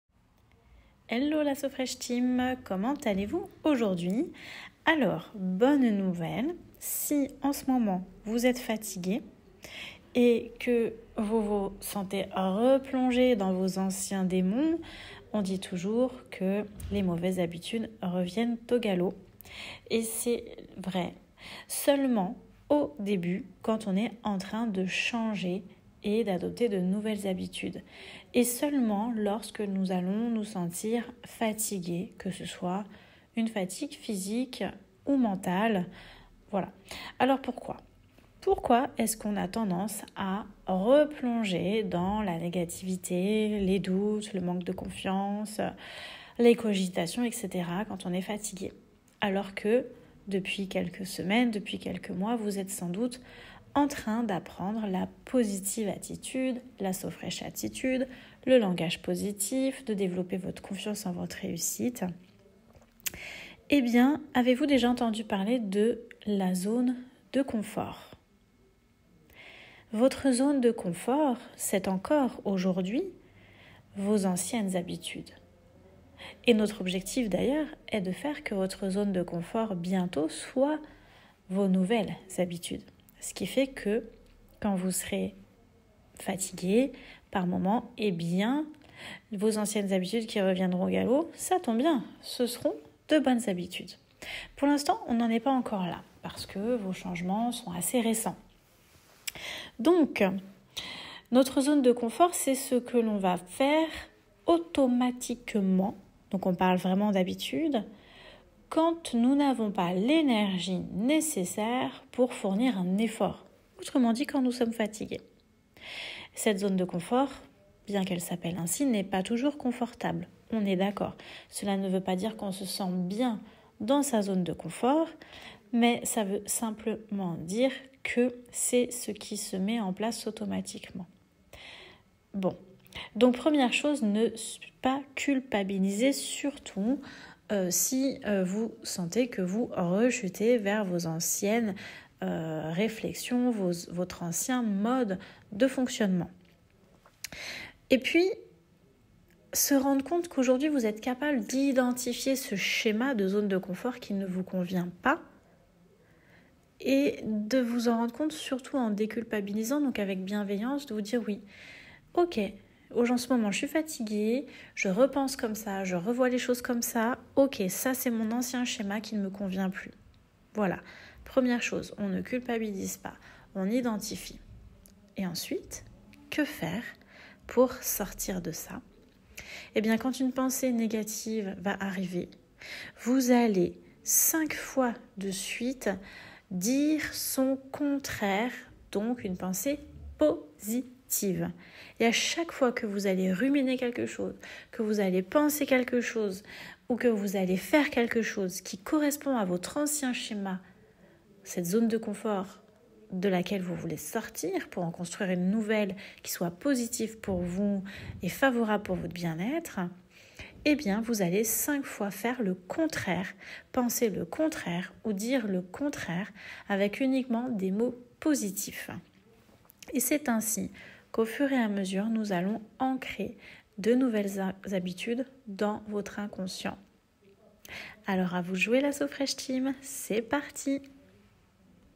La zone de confort : un automatisme à maitriser pour réussir votre amincissement durablement
Vous remarquerez que dès que vous êtes dans une période de fatigue, qu'elle soit une fatigue physique ou une fatigue nerveuse, vos anciennes habitudes ont tendance à revenir au galop, et bien c'est normal !

Cette zone de confort peut être aussi inconfortable, et pourtant, elle représente notre schéma d'habitudes et d'automates.

Avec votre accompagnement So Fresh & Diet, vous apprenez à penser positif et à nourrir la confiance en la réussite de vos objectifs. Cela vous demande un effort de modifier votre état d'esprit.
En cas de fatigue, fournir un effort devient inconsciemment quelque chose de difficile, et les priorités de la vie prennent le dessus, alors vos pensées négatives et anciennes habitudes alimentaires peuvent vite refaire surface.

Dans ce podcast je vous explique comment sortir progressivement de ce schéma pour commencer à avancer définitivement, pas à pas, périodes de fatigue après périodes de fatigue, vers votre poids idéal.

🍑 Mes partenaires bienêtre : 🍑 @Le Pavé de la Halle 🍑 @evidemment soi 🍑 @Aurélia Ryckebusch - Thérapeute Holistique 🍑 @Synda Percez’verrez 🍑@Infertiline 🍑 @JA Coaching Sportif 🍑